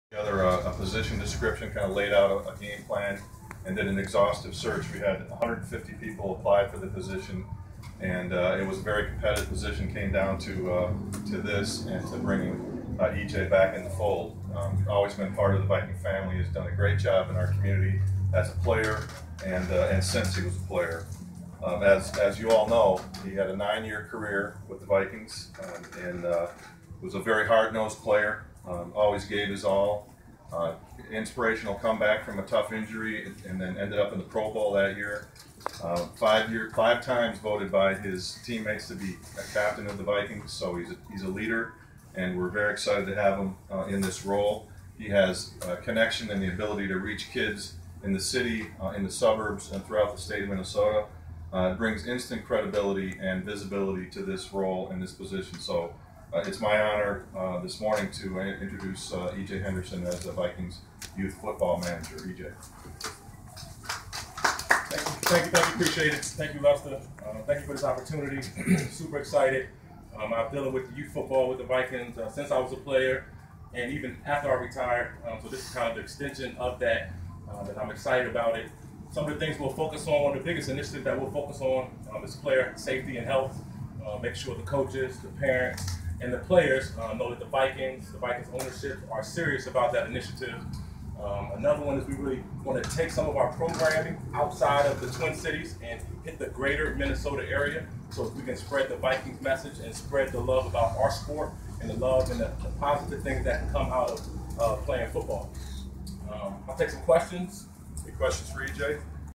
EJ Henderson on his new position with the Vikings
Lester Bagley introduces E.J. Henderson, the Vikings new manager of youth football.